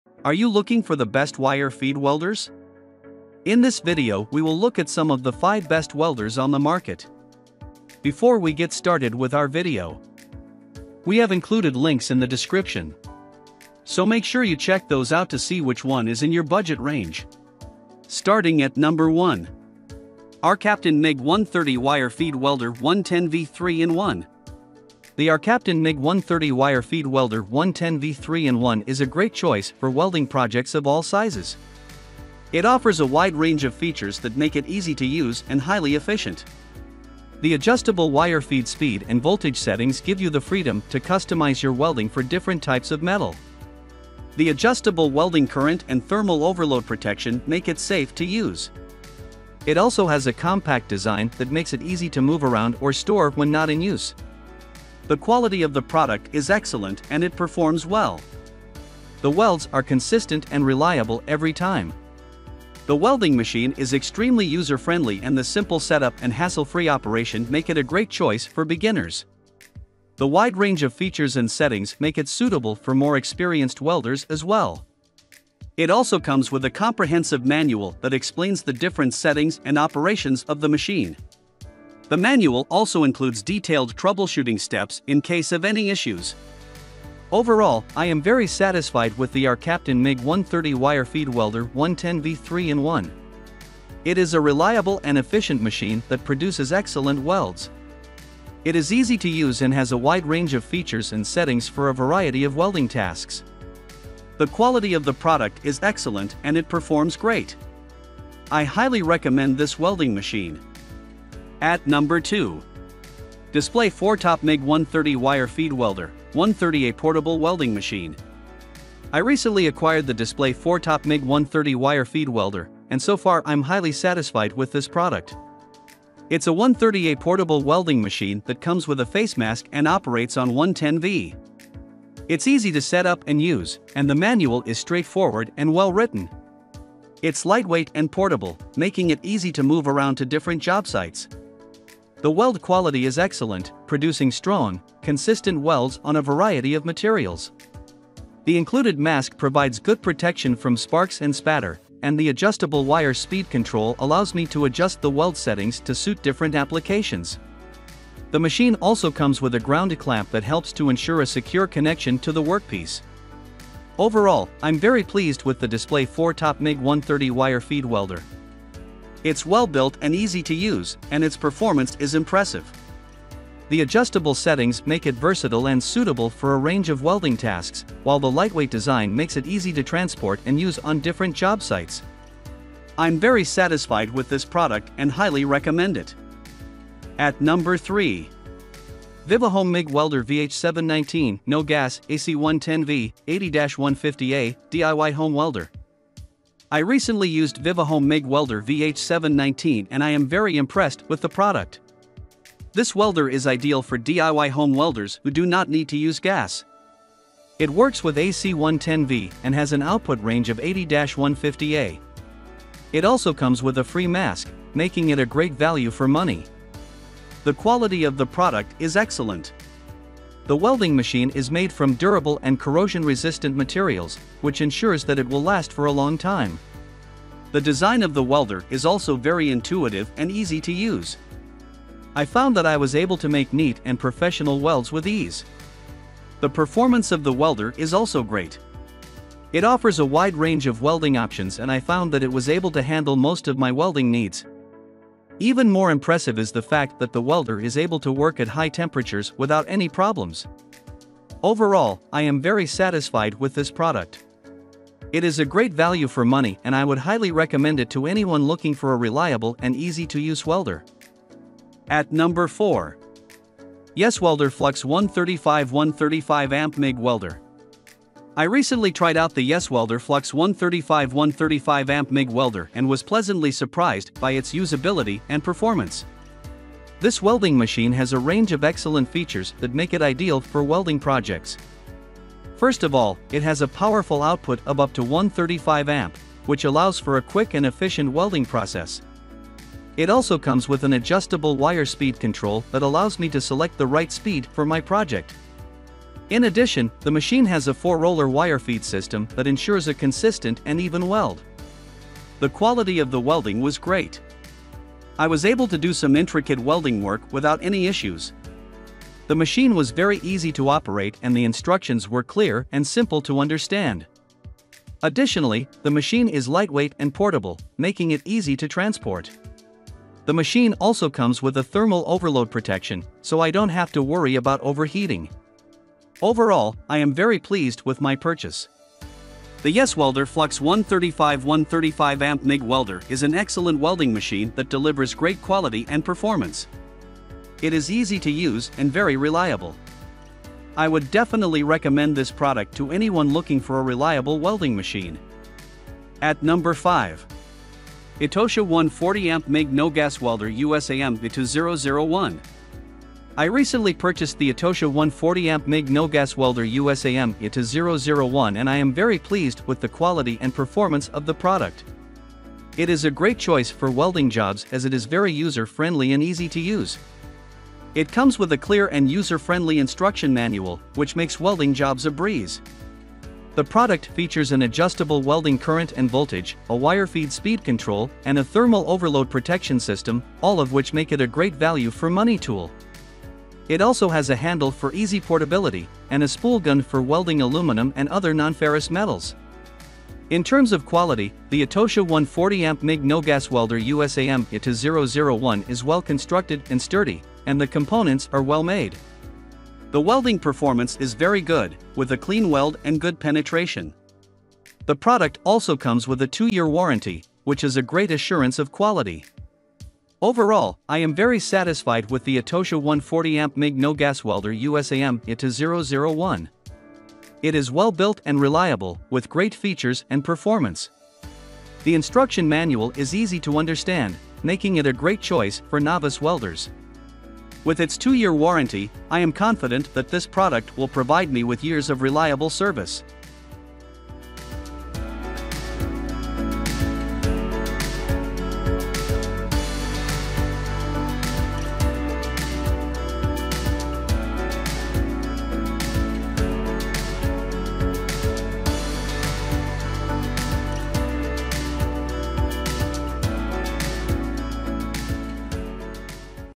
✅ Best Wire Feed Welders 2023 - Top 5 Wire Feed Welders Review - Top Rated Wire Feed Welders 2023
In this video, we listed the Best Wire Feed Welders that are available on the market for their true quality, actually, I tried to make the list based on their popularity quality-price durability user opinions and more.